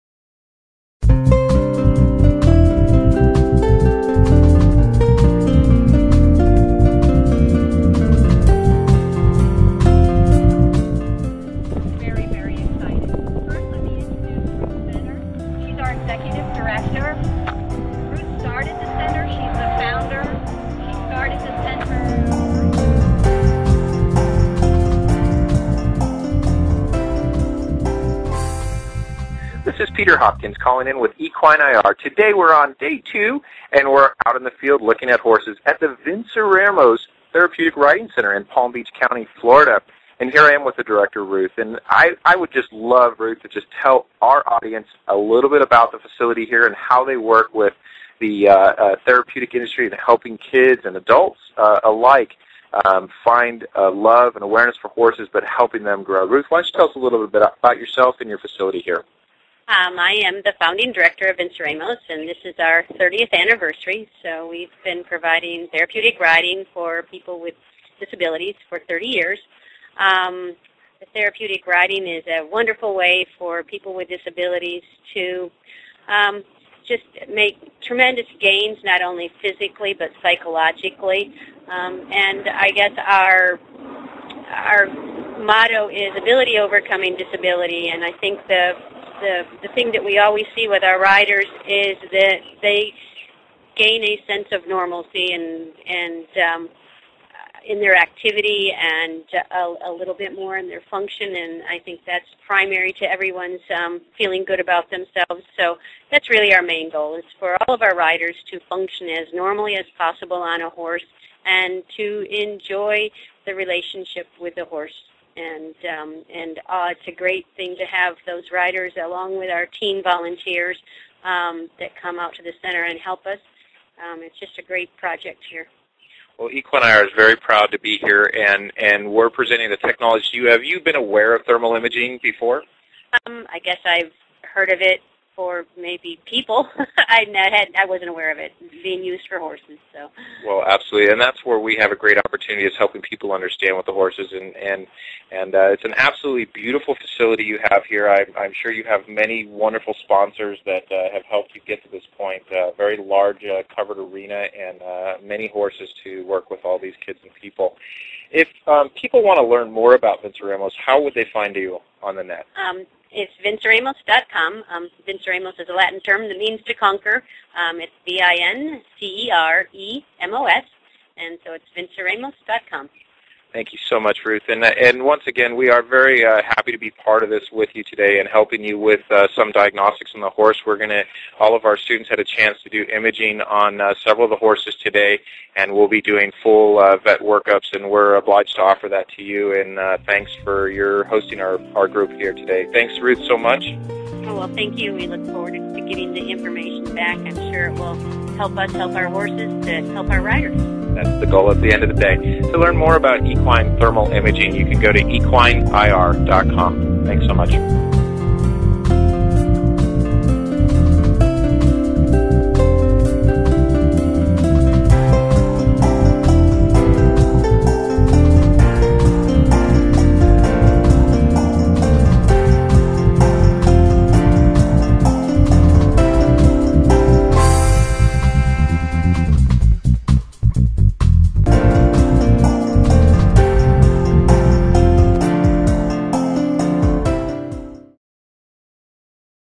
Vinceremos Therapeutic Riding Center, Palm Beach, FL - EquineIR Field Trip - 9 March 2012
- In Latin, "Vinceremos" means to overcome. Using gentle movement of the horse as therapy and recreation, Vinceremos Therapeutic Riding Center program serves to enhance the lives of special needs people in Palm Beach County. EquineIR made a visit to Vinceremos on 9 March 2012 as part of its Florida training class. Ruth Menor, the executive director, talks with Peter Hopkins of EquineIR. Video by Ken English,